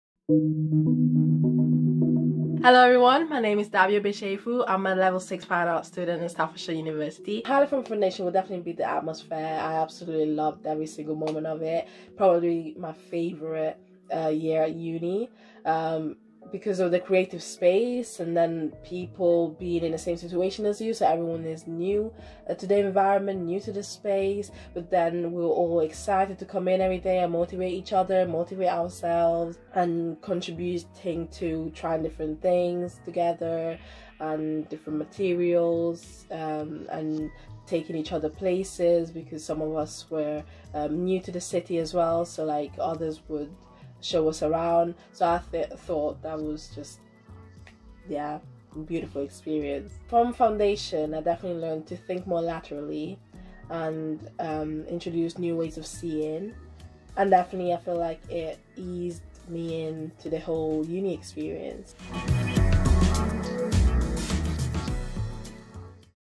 Why I chose Foundation Art and Design at Staffs
Fine Art graduate, Daby Obiechefu talks about the creative freedoms and welcoming environment that ensured the Art and Design Foundation year got her ready for degree study.